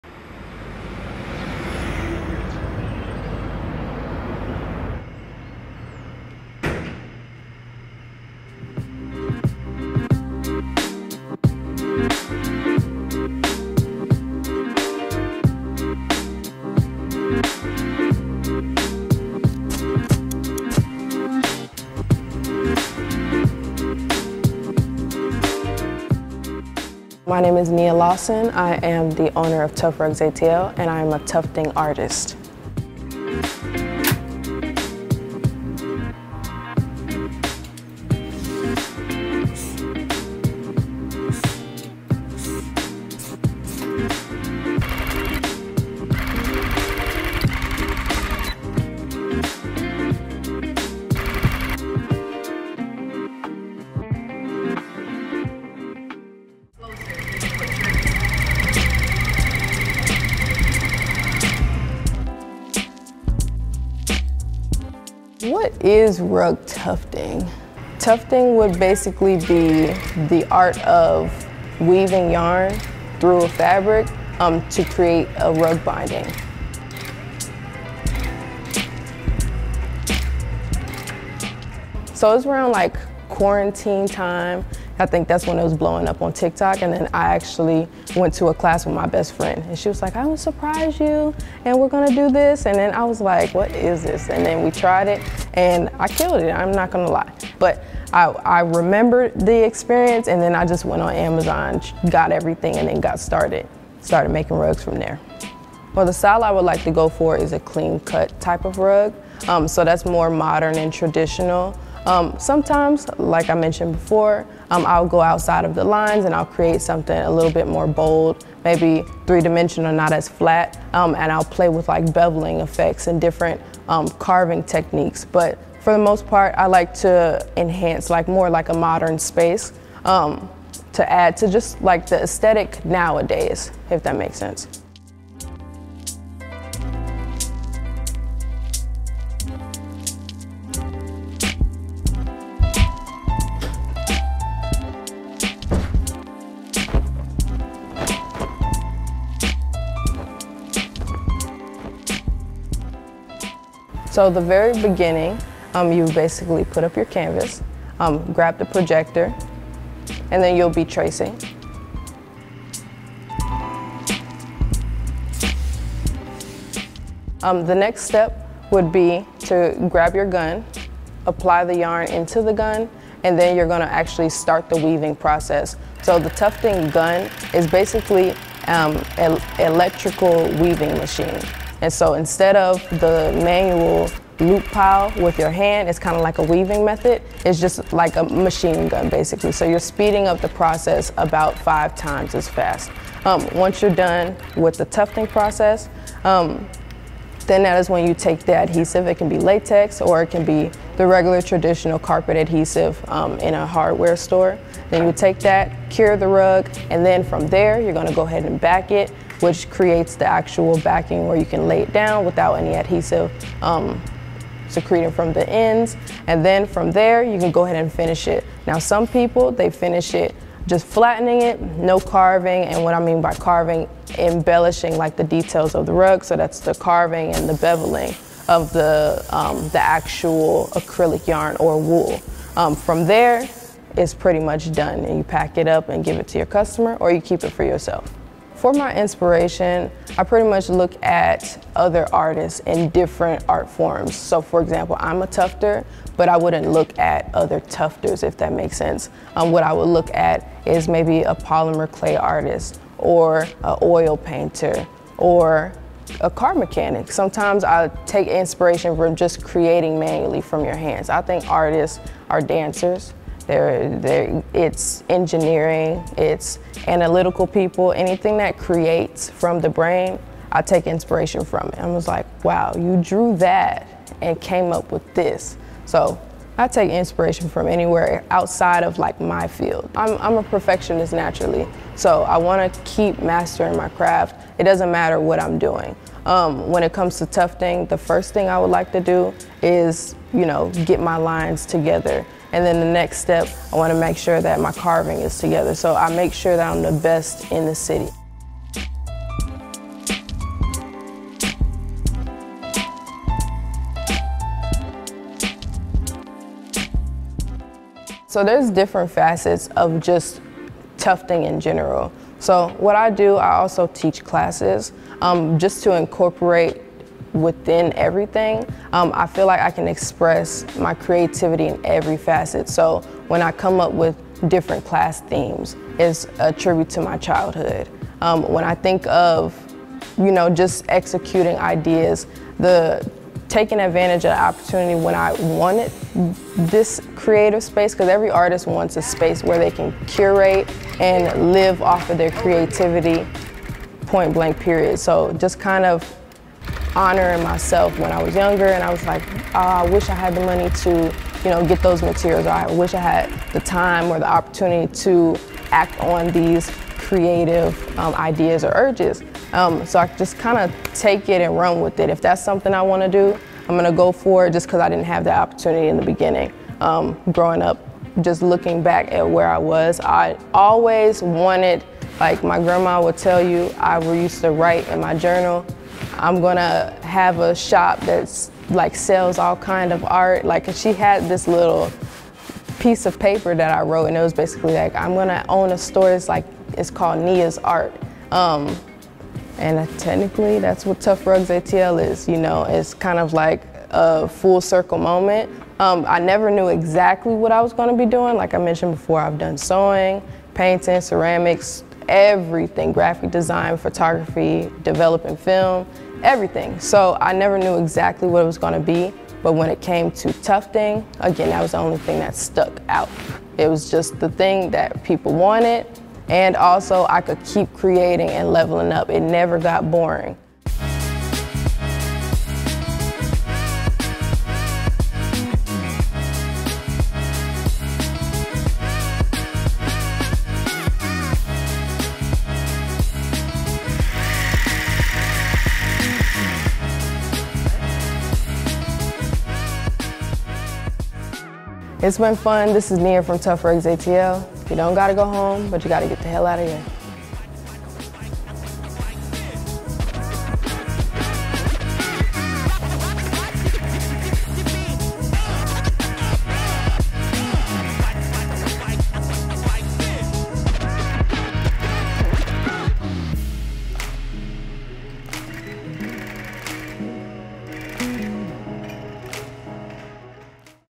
Art Comes Alive | Tuft Rugs ATL
From yarn to art, Nia Lawson of Tuft Rugs ATL transforms fibers into bold, clean-cut rugs. This trending medium of rug tufting has taken the internet by storm, combining personality, creativity, and utility to create stunning home decor pieces. Join Nia as she guides us through her studio, showcasing her craft and process. Discover the techniques, tools, and inspiration behind each unique piece, and let your creativity be ignited.

Be sure to follow AIB on social media!